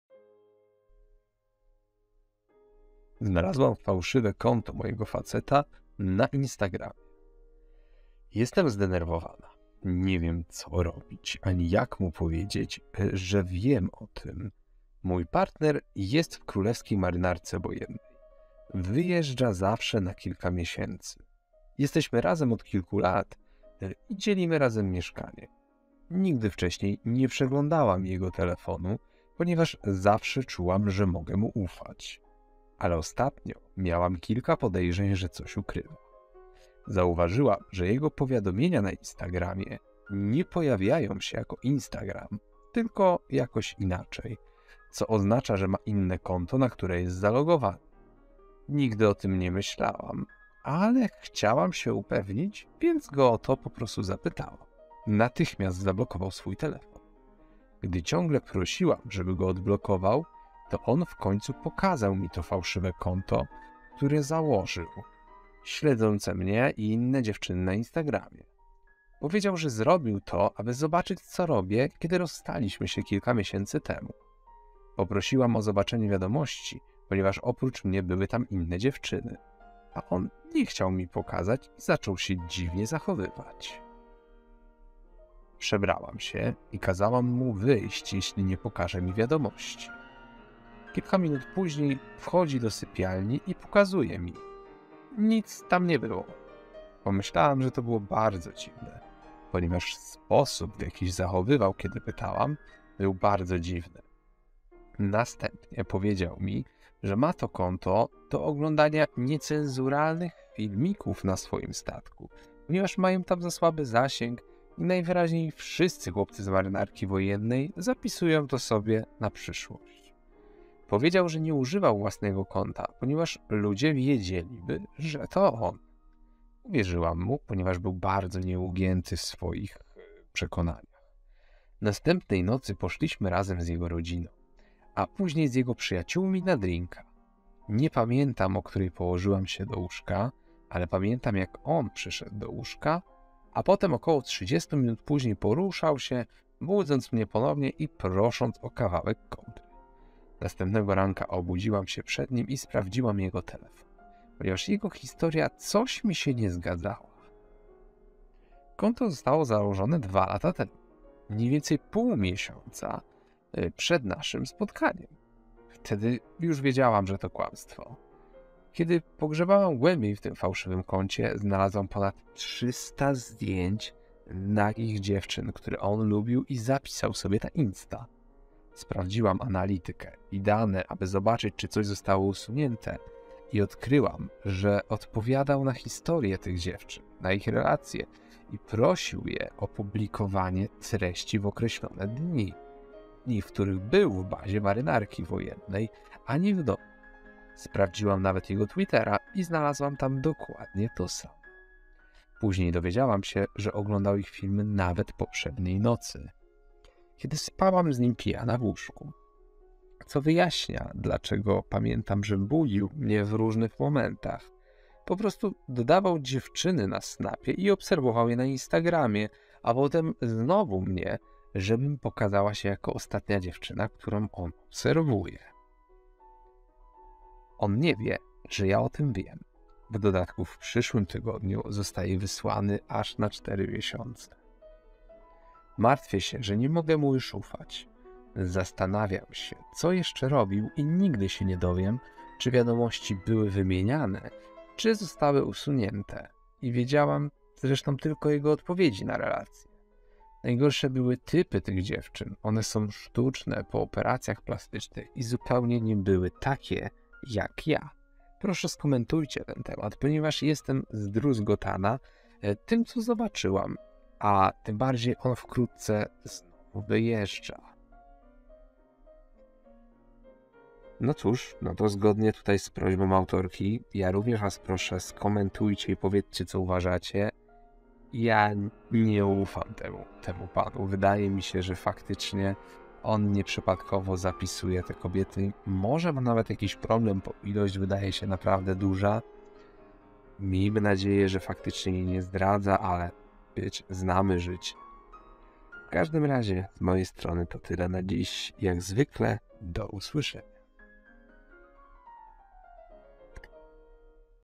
Znalazłam Fałszywe Konto Na Instagramie Mojego Faceta - Reddit Podcast
Fałszywe konto na Instagram? To musi być zdrada!

Czekam na Wasze komentarze!

👍 Proszę o łapkę w górę, jeżeli Ci się spodobało!
00:00 - Pierwsza historia
04:30 - Opinia